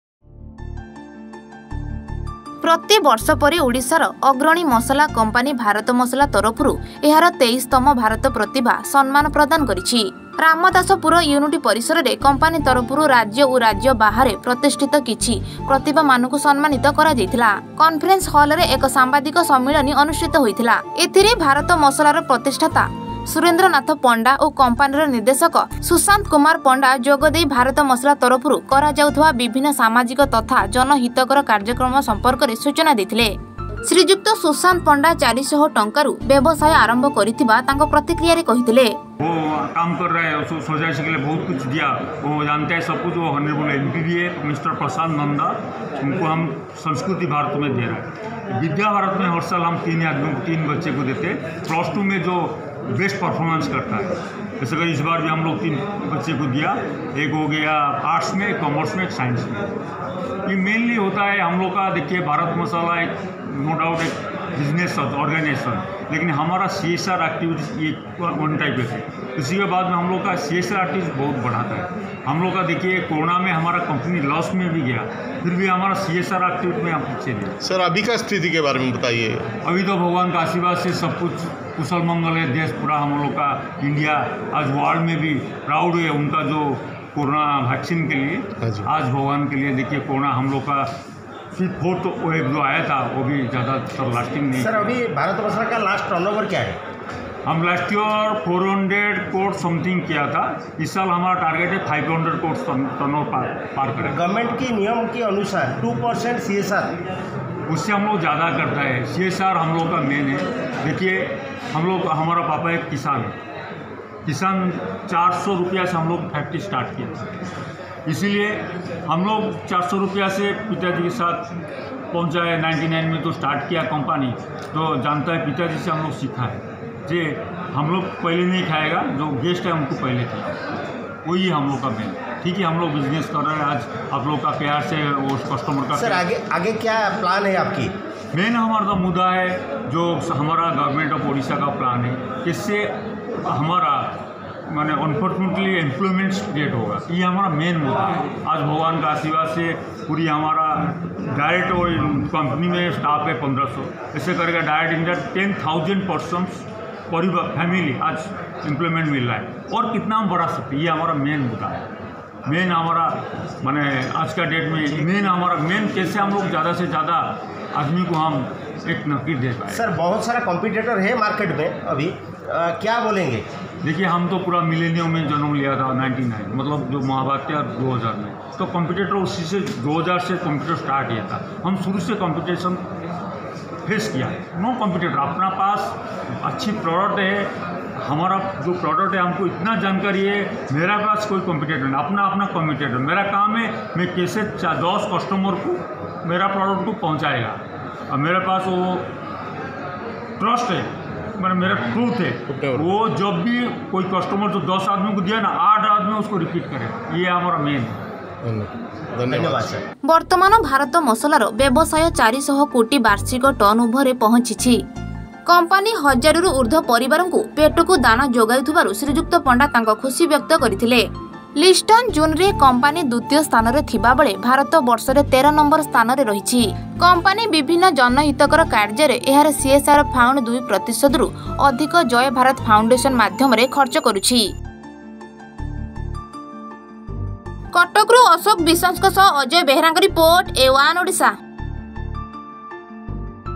ଭାରତ ମସଲାର 23ତମ ବାର୍ଷିକ ଉତ୍ସବ ପାଳନ | A1 ODISHA
ପ୍ରତି ବର୍ଷ ପରି ଓଡ଼ିଶାର ଅଗ୍ରଣୀ ମସଲା କମ୍ପାନୀ ଭାରତ ମସଲା 23ତମ ବାର୍ଷିକ ଉତ୍ସବ ପାଳନ।